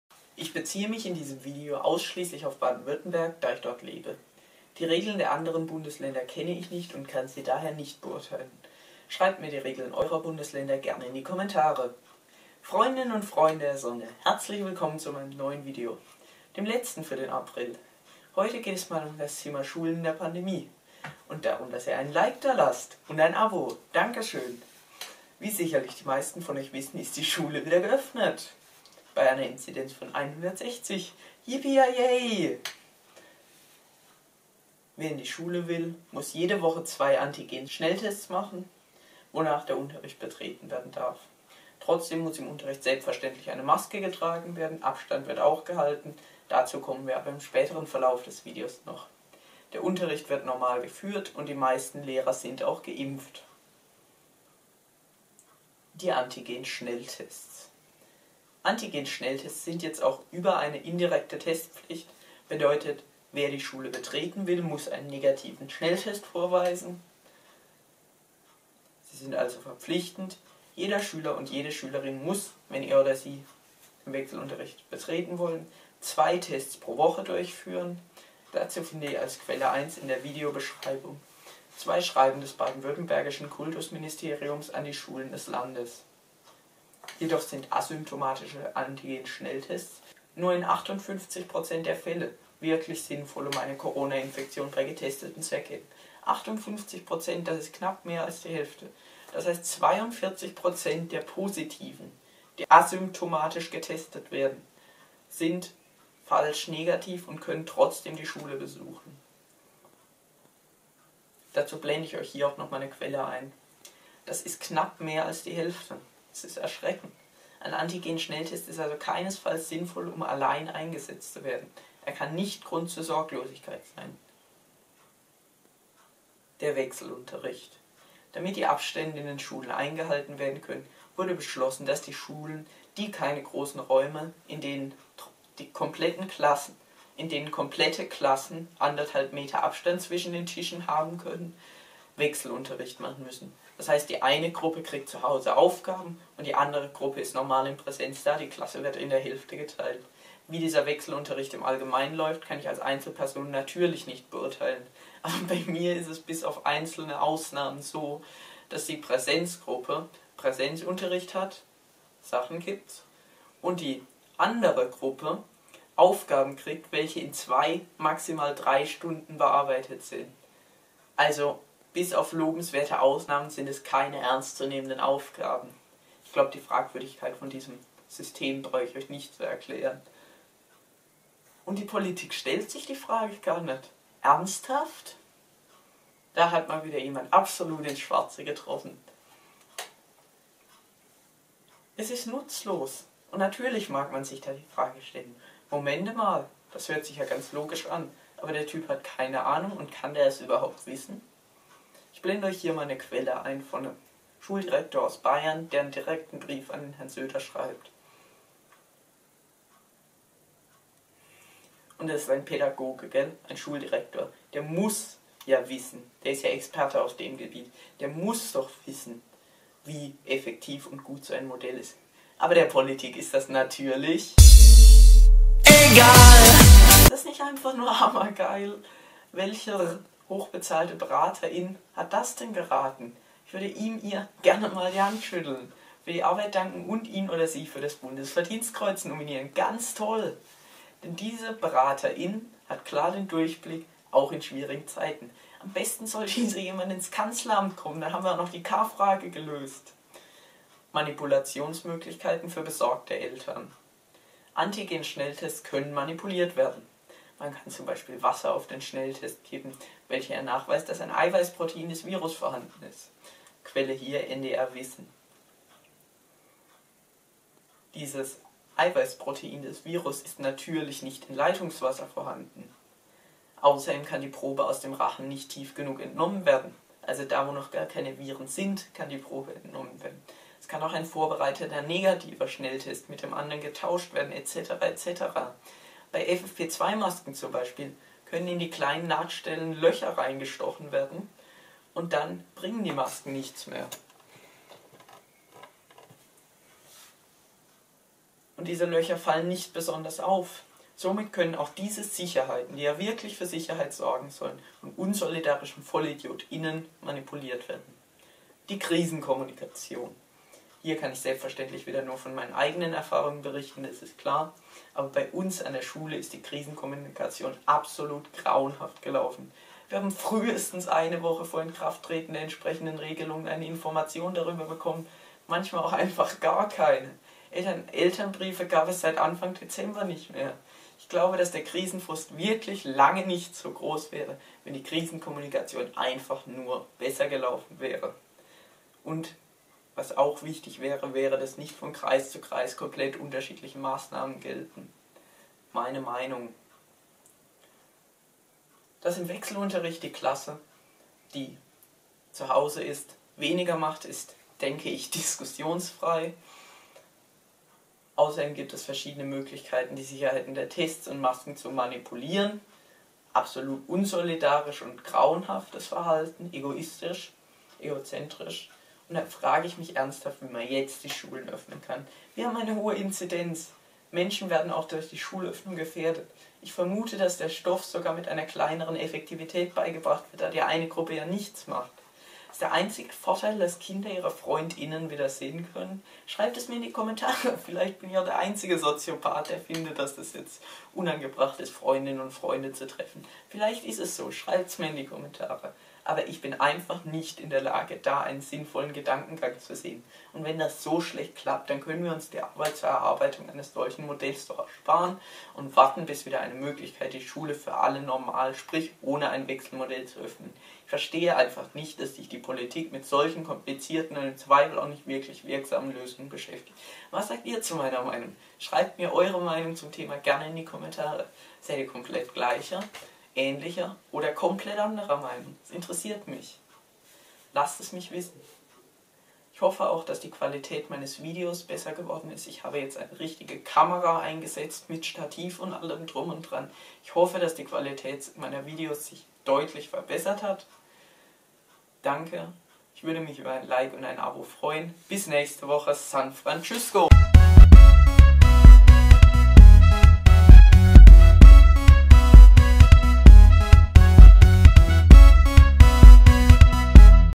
Das Versagen der Schule in der Pandemie
Hallo, herzlich willkommen zu meinem neuesten Video, heute geht es mal um das Thema Schule in der Pandemie.
                                                               Quellen
                                                                      Equipment
Kamera: Panasonic® LUMIX DMC-LF1
Schnittprogramm: Adobe® PremierePro 2020
Bildbearbeitung: Adobe® PhotoShop 2021
Wenn ihr möchtet, könnt ihr auch gerne auf das Video reagieren.